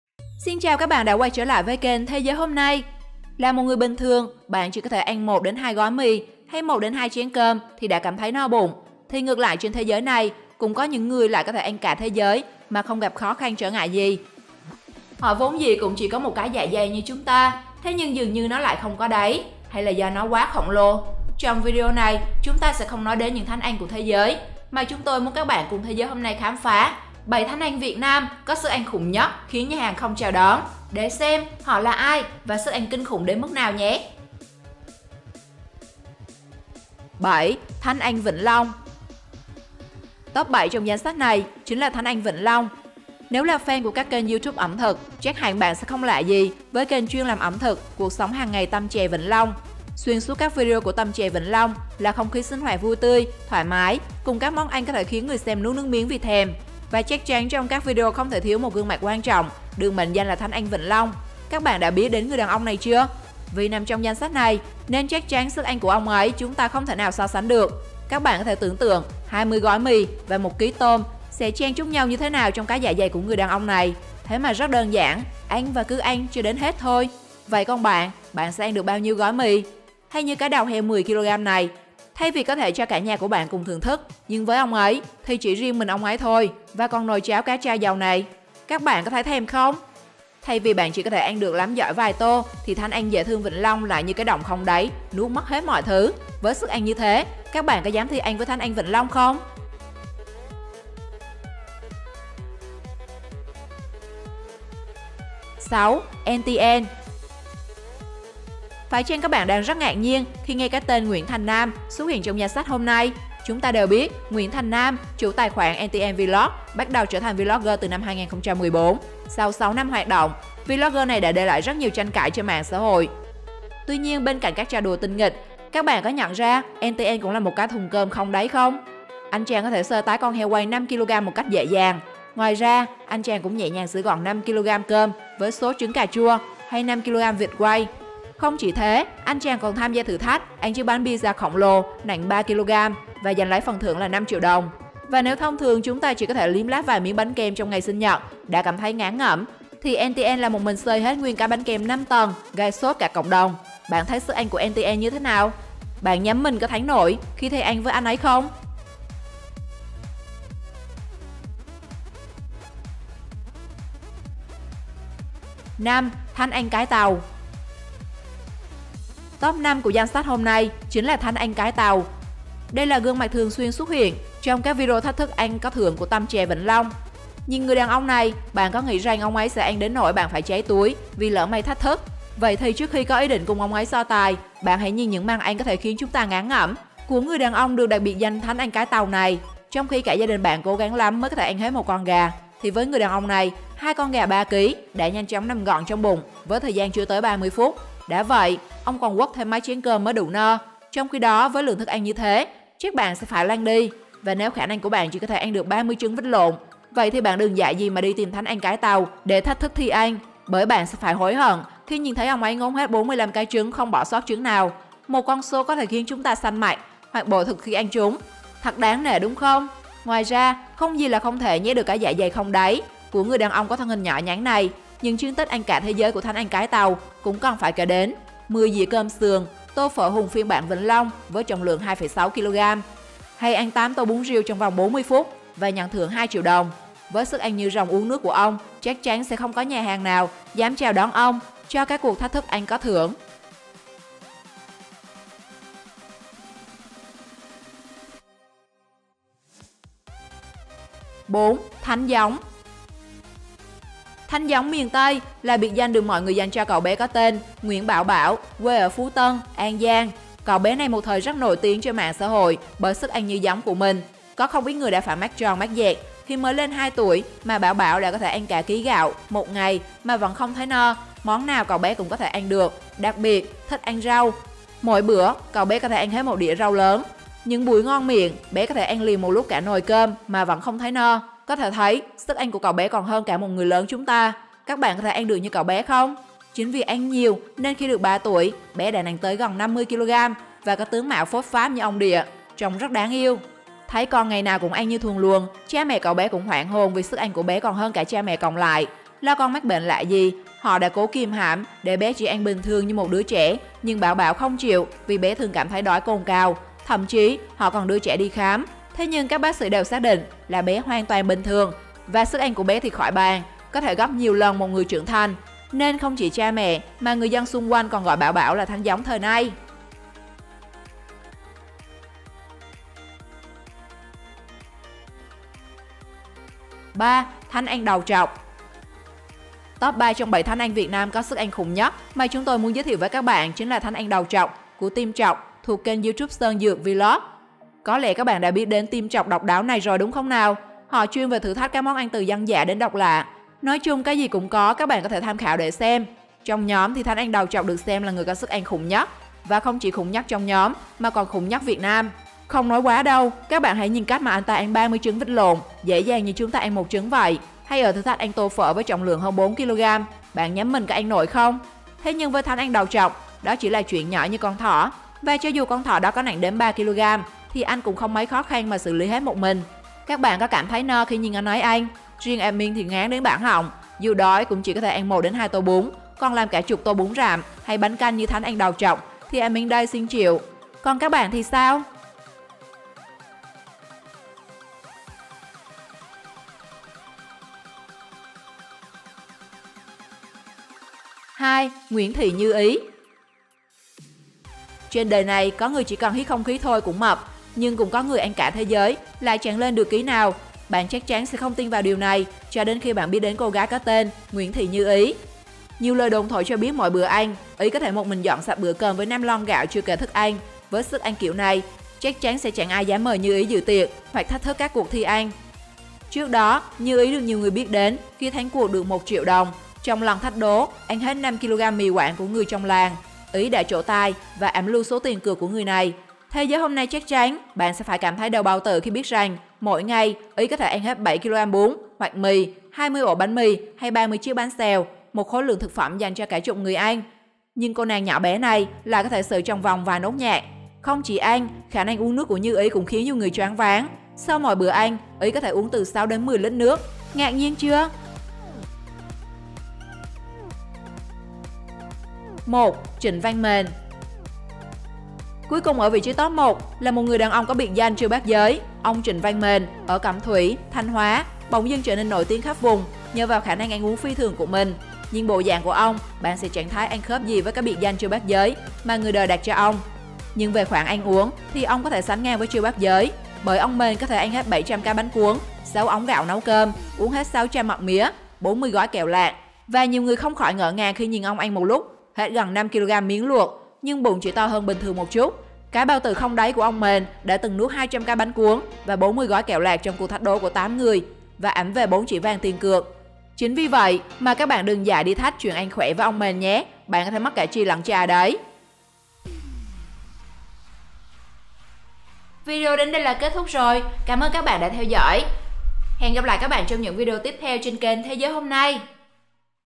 Mỗi Lần Ăn 20 Gói Mì Không No | Top 7 Thánh Ăn Việt Nam Có Sức Ăn Khủng Nhất Việt Nam
Một số tư liệu video được cung cấp bởi: @NTNVlogsNguyenThanhNam , @tamchevinhlong , @amthucdongqua91512

Chào các bạn, là một người bình thường bạn chỉ có thể ăn 1 đến 2 gói mì hay 1, 2 chén cơm thì đã cảm thấy no bụng, thì ngược lại trên thế giới này cũng có những người lại có thể ăn cả thế giới mà không gặp khó khăn trở ngại gì. Họ vốn vĩ cũng chỉ có một cái dạ dày như chúng ta, thế nhưng dường như nó lại không có đáy, hay là do nó quá khổng lồ. Trong video này chúng ta sẽ không nói đến những thánh ăn của thế giới, mà chúng tôi muốn các bạn cùng Thế giới hôm nay khám phá 7 Thánh Ăn Việt Nam Có Sức Ăn Khủng Nhất Khiến Nhà Hàng Không Chào Đón, để xem họ là ai và sức ăn kinh khủng đến mức nào nhé. NTN có thể ăn cả 1 con heo sữa quay.

00:00 Giới thiệu
00:37 7. Thánh ăn Vĩnh Long
01:45 6. NTN
02:45 5. Thánh Ăn Cái Tàu
04:10 4. Thánh Gióng
05:26 3. Thánh Ăn Đầu Trọc
06:31 2. Nguyễn Thị Như Ý
07:32 1. Trịnh Văn Mền